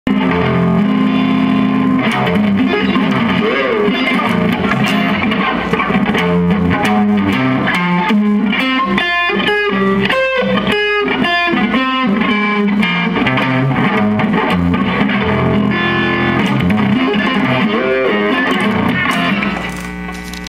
Common Minor Sweep Picking Lick With Extention - Drop C tuning.
I have noticed this minor sweep with the baby finger slide at the end is used a lot in metal and other music. It's a cool little trick.